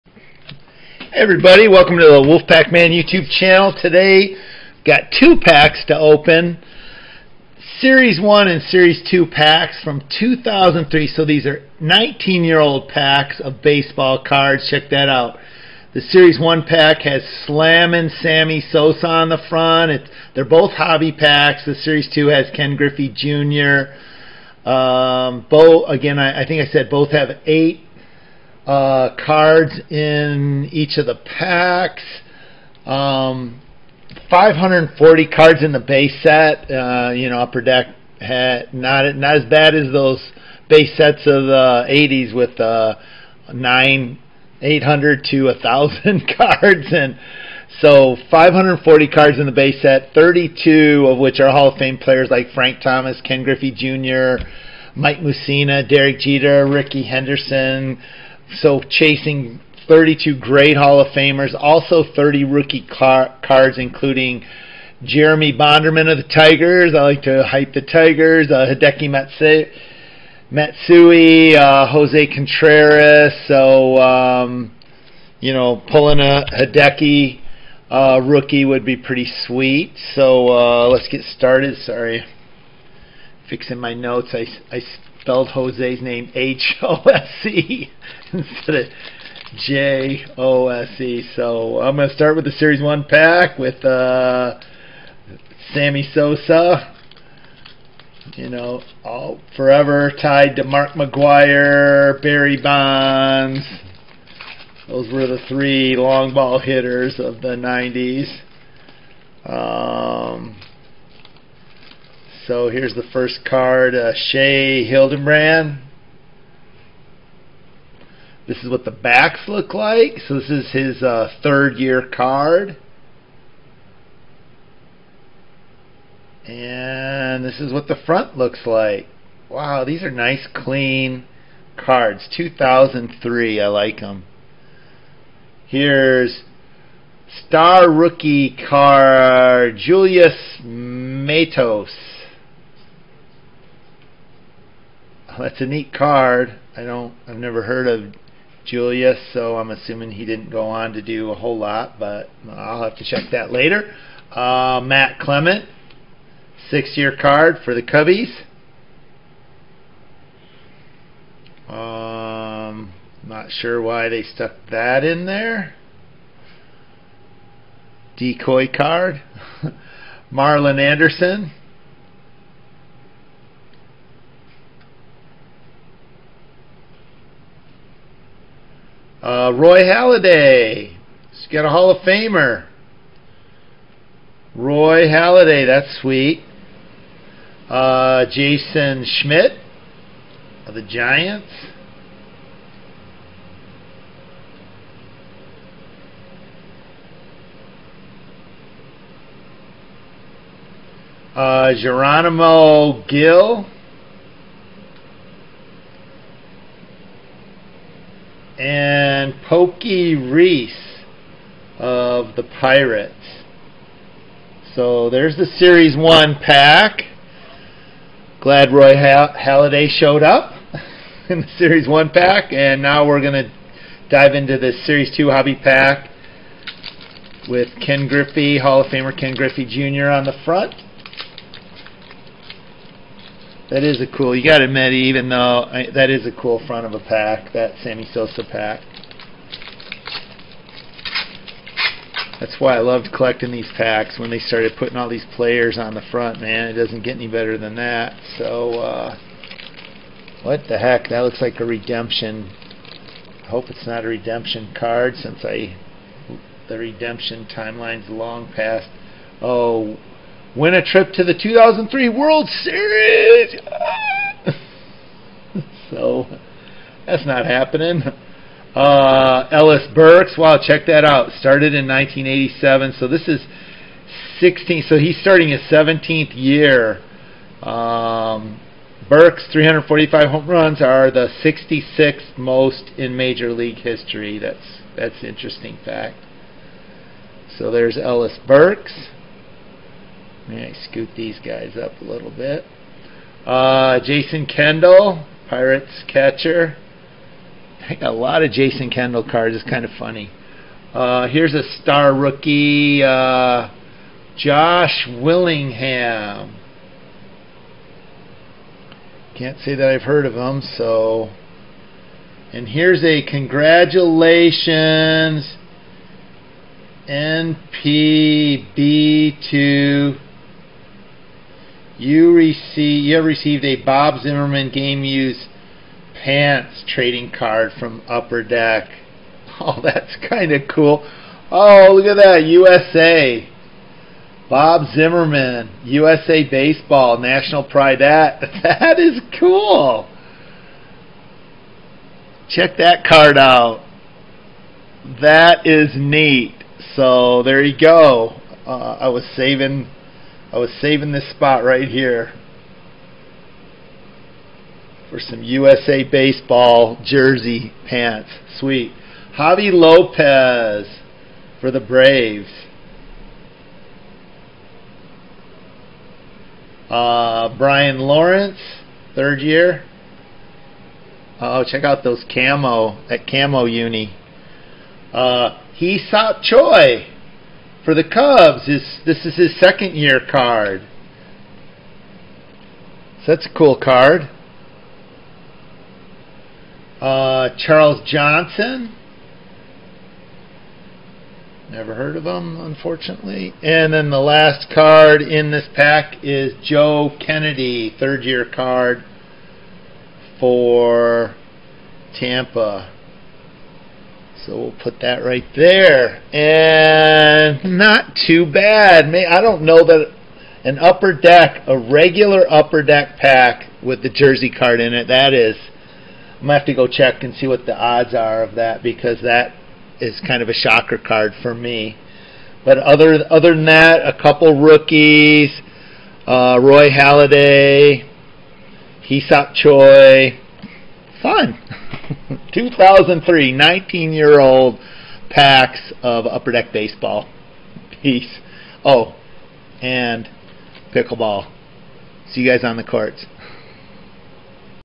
2003 UPPER DECK BASEBALL pack rip with a TEAM USA jersey card plus Roy Halladay and Hee Seop Choi!
2 packs of 2003 Upper Deck baseball cards.

The series 1 hobby pack has Sammy Sosa on
the front with 8 cards.
The series 2 hobby pack also has 8 cards
and Ken Griffey Jr. on the front.

The base set has 540 cards
32 of which are hall of fame players like
Frank Thomas,
Ken Griffey Jr.
Mike Mussina,
Derek Jeter,
Rickey Henderson.

Also 30 rookie cards including
Jeremy bonderman of the Tigers,
Hideki Matsui,
Hose Contreras,

Found: Bob Zimmerman USA jersey card, plus Roy Halladay,
Hee Seop Choi 2nd year card,
and Javy Lopez!